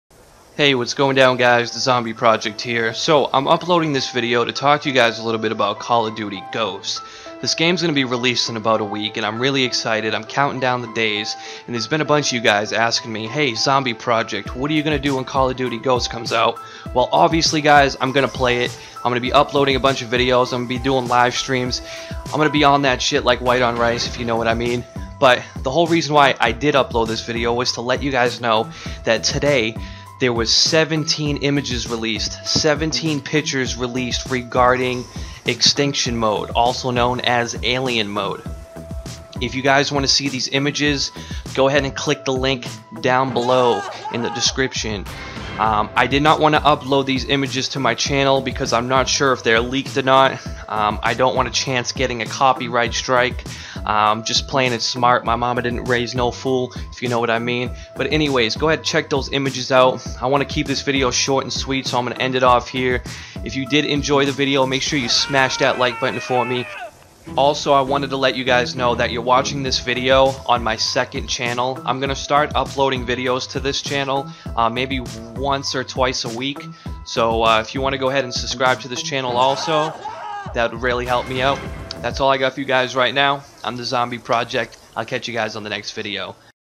Call Of Duty Ghost: Alien Mode 17 Photos "Extinction Mode Gameplay Photo's"
Call Of Duty Ghost: Alien Mode 17 Photos "Extinction Mode Gameplay Photo's" Ghost Alien Mode Gameplay Snapshot's
I herd you will have to Complete the Call Of Duty Ghost Campaign to unlock Alien / Extinction Mode! If this is true i will be live streaming the Full Campaign on Release Date (11/05/2013)